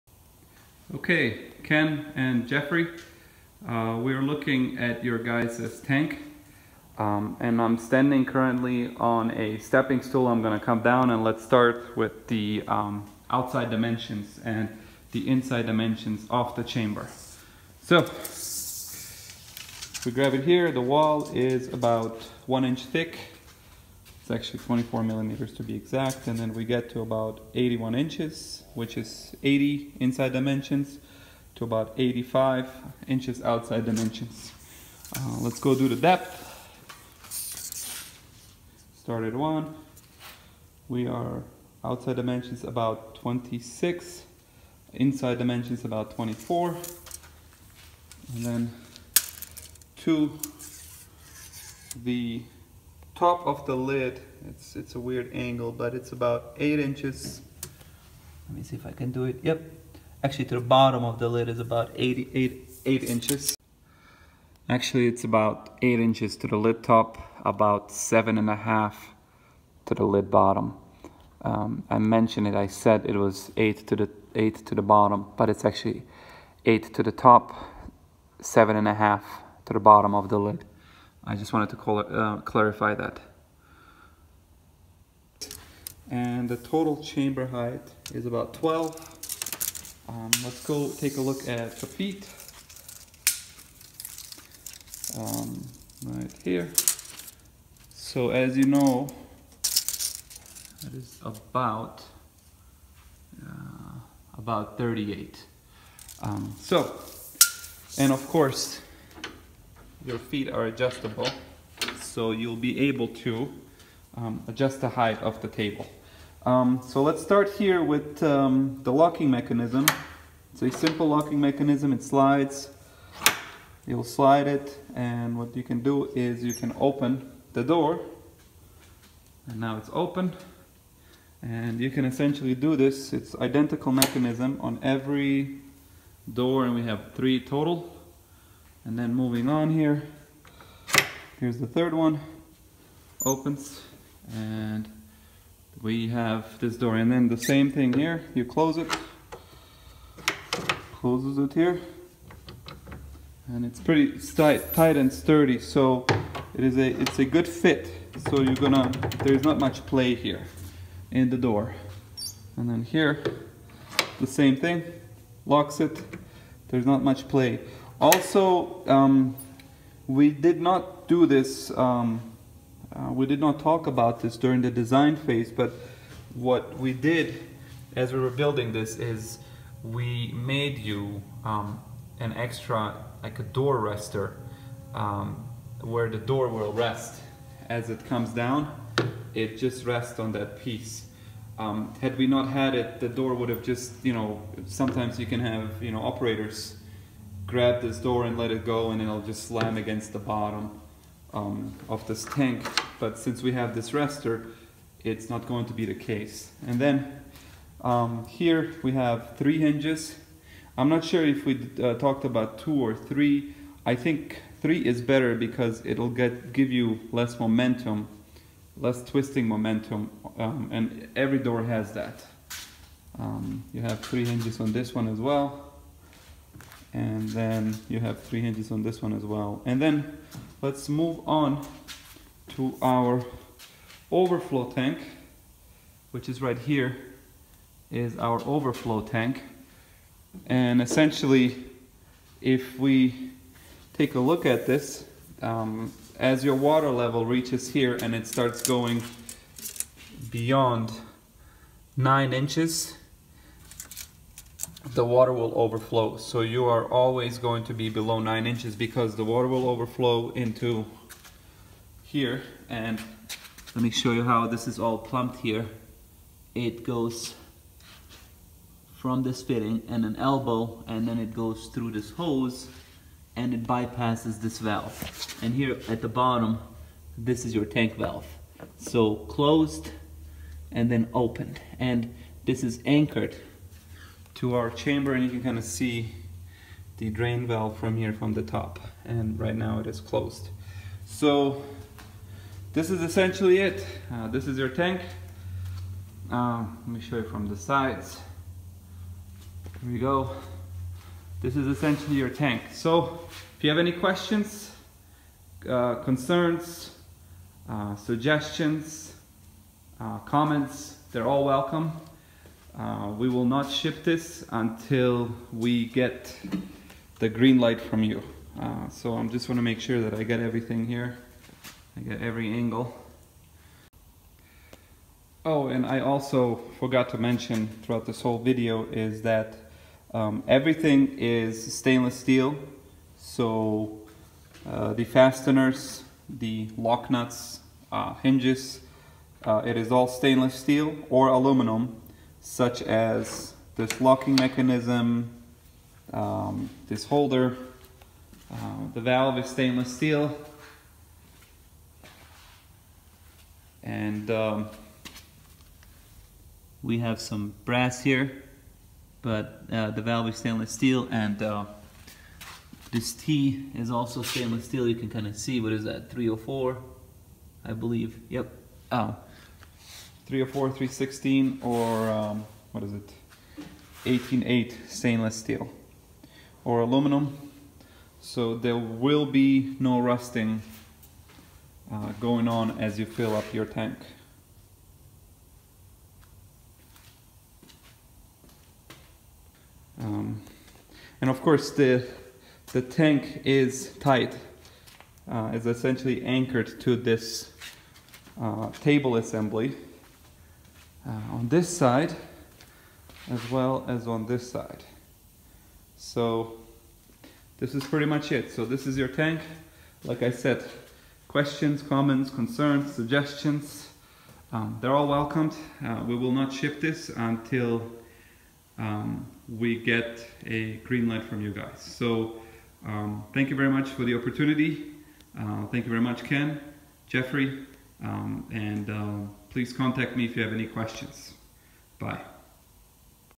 ASTM F2096 Submersion Leak Test Tank for Detecting Gross Leaks in Packages (Pressure Bubble Test)
ASTM F2096 Submersion Leak Test Tank for Detecting Gross Leaks in Packaging by Internal Pressurization (Bubble Test)